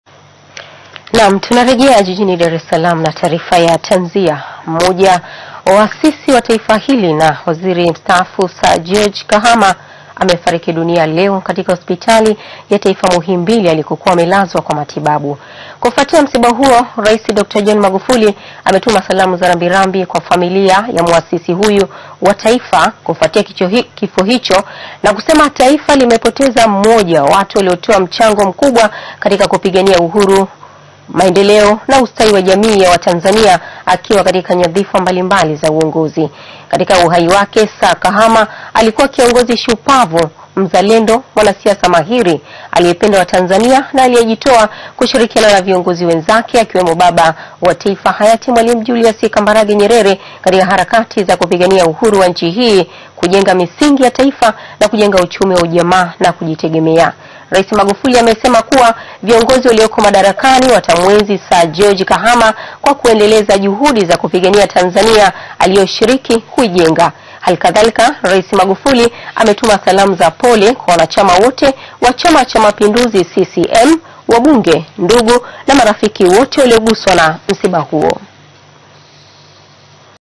Sir George Kahama Afariki Dunia
Mmoja wa waasisi wa Taifa na waziri mstaafu Sir George Kahama, amefariki dunia hii leo katika Hospitali ya Muhimbili alikokuwa akipatiwa matibabu